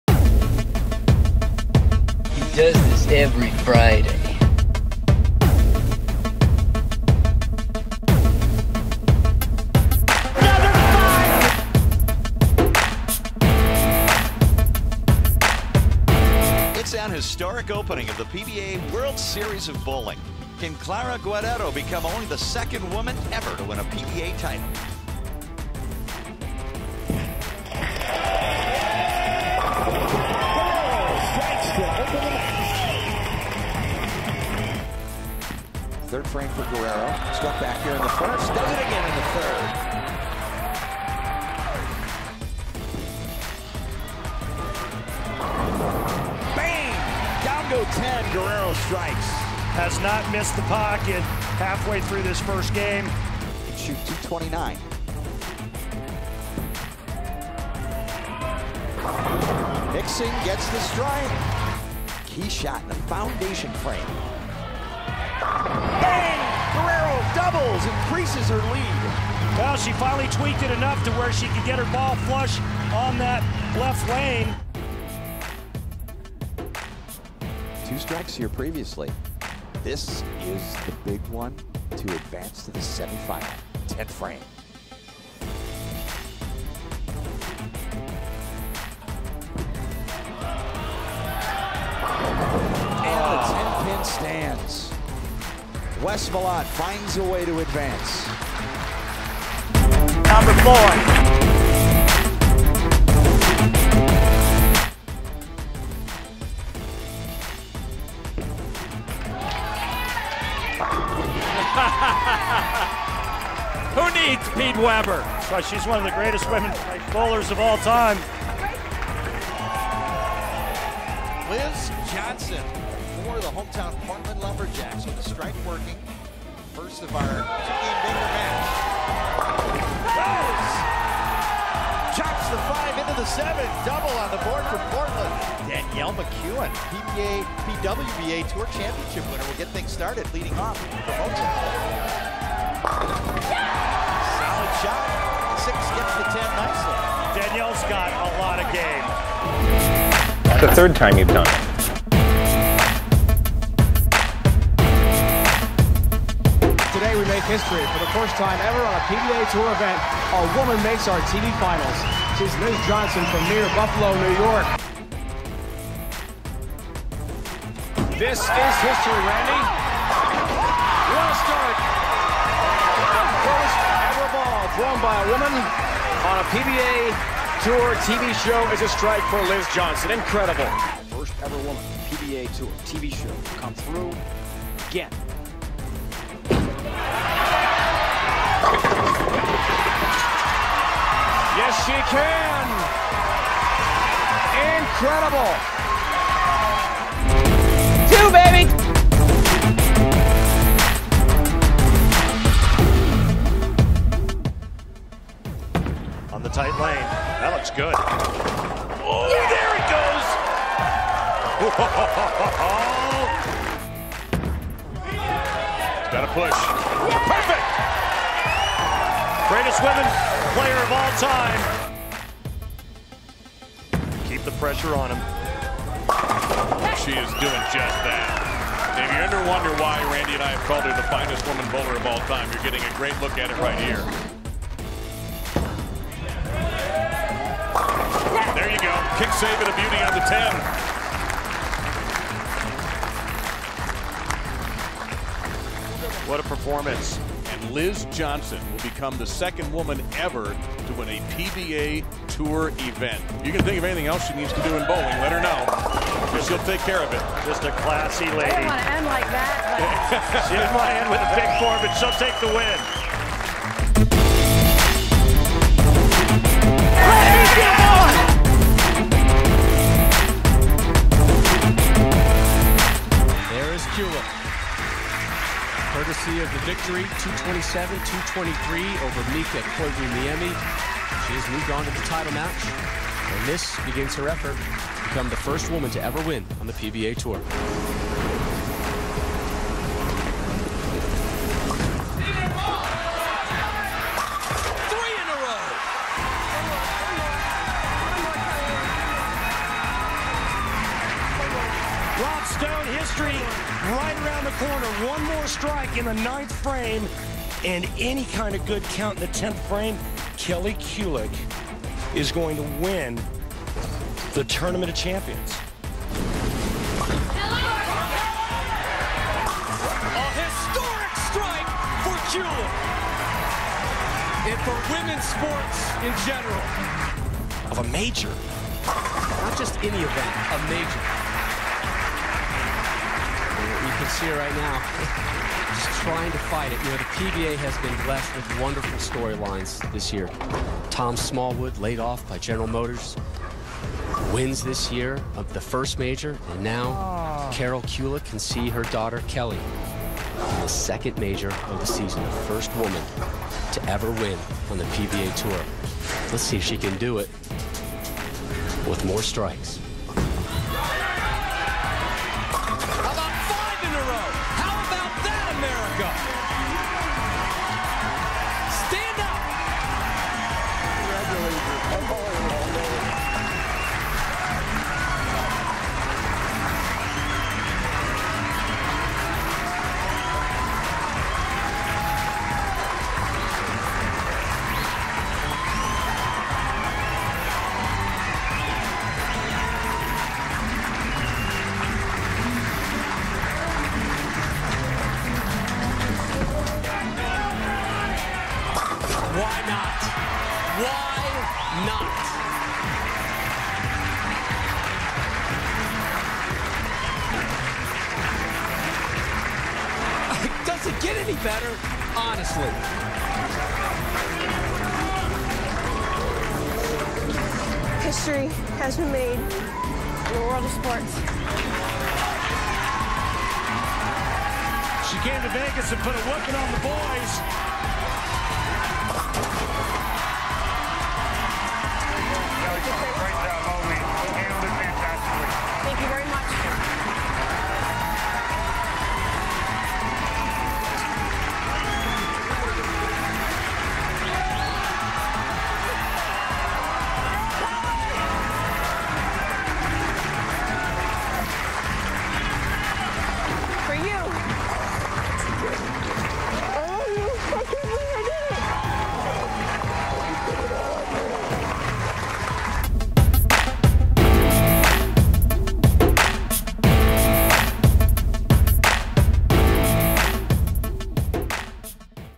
Friday Five - Best Performances by Women in PBA History
Friday Five, June 26, 2020: some of the best moments from the top female bowlers in PBA history.

With the recent announcement of two new all-female PBA League teams for the 2020 season, we celebrate some of the notable performances by women in the PBA.

Three women–Kelly Kulick, Liz Johnson and Danielle McEwan–have already appeared in PBA League competition. Clara Guerrero finished fourth in the 2013 Cheetah Championship. Johnson was the first woman to make a PBA TV show in the 2005 Banquet Open, finishing second after losing a close title match to Tommy Jones. Johnson later became the second woman to win a PBA Tour title when she defeated Anthony Pepe in the 2017 Chameleon Championship.

Most famously, Kulick defeated Chris Barnes in the title match of the 2010 Tournament of Champions. Kulick became the first woman to win a PBA Tour title and a major at that.